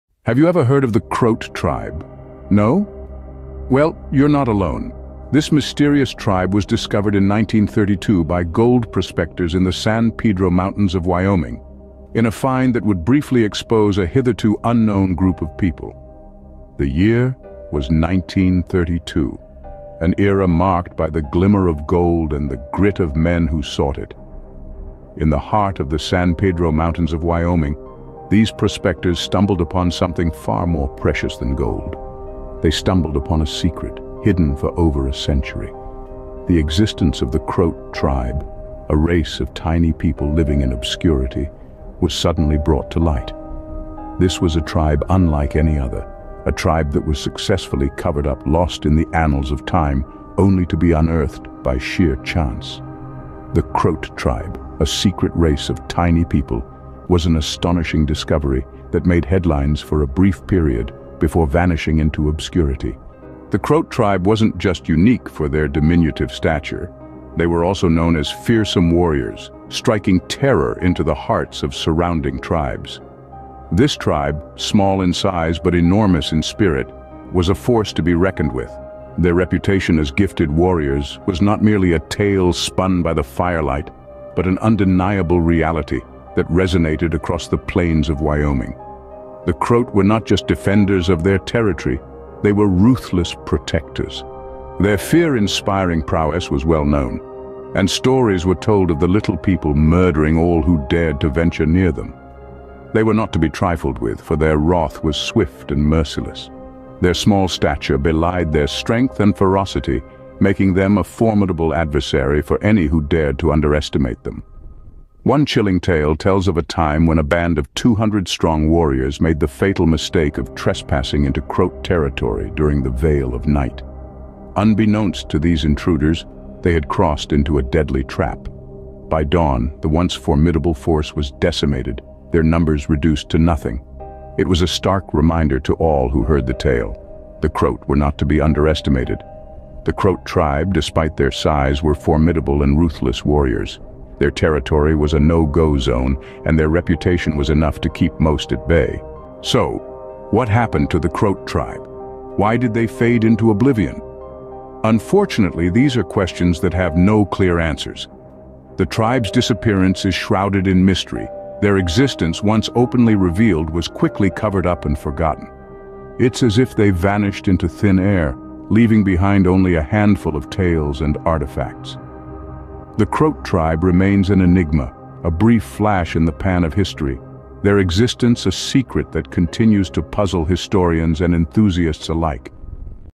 The Missing Ancient Warriors
Get ready to uncover the secrets of The Hidden Warriors as we unveil the epic battle against the fearsome Crote! Brace yourselves for an action-packed adventure that will leave you on the edge of your seat. Join us as we dive deep into the untold stories, unmasking their true strength and revealing the legends behind these extraordinary warriors. Prepare to be amazed by their unmatched skills and indomitable spirit.